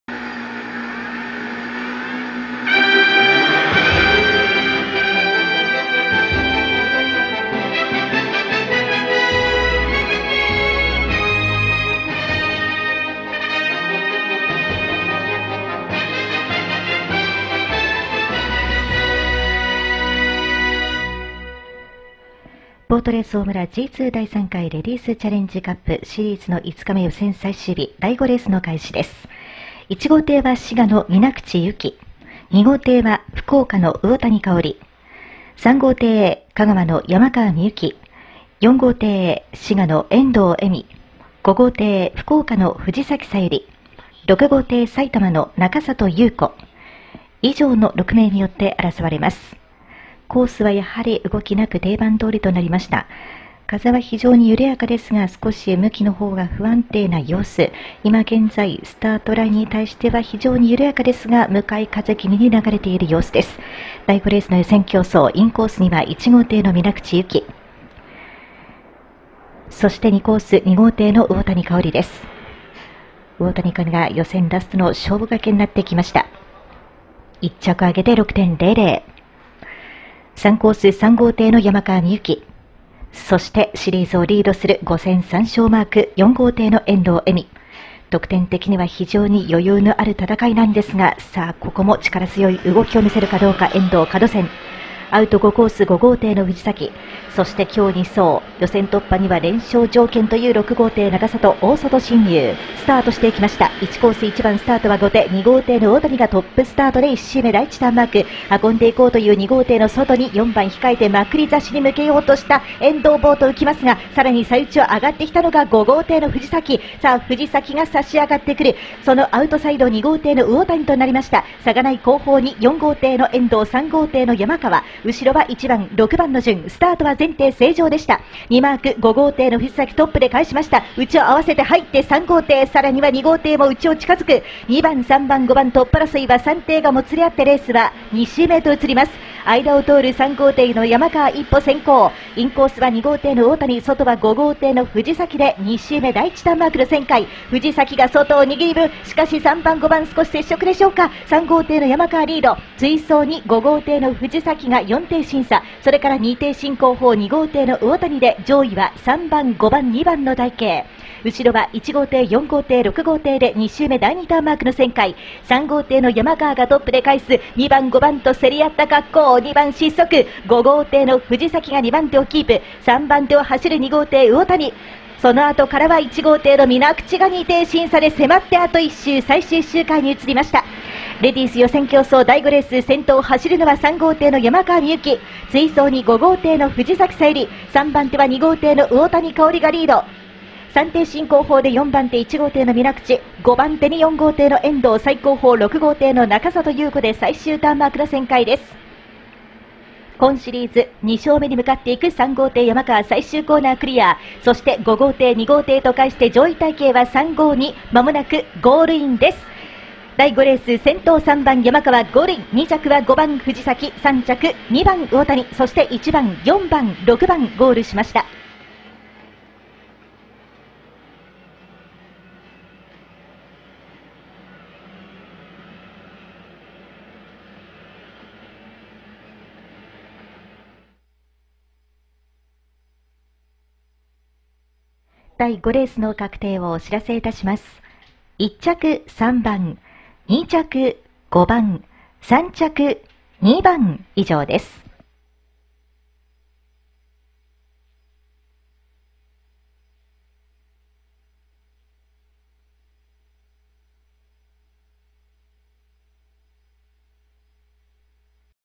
チャレンジカップ／Ｇ２レディースチャレンジカップ ５日目 5R 女子レースのTOP争いがヤバすぎる！！
希少映像！！このレースが凄い！！！最新レース動画特集！！

BRF当日予想ブログ公開中！！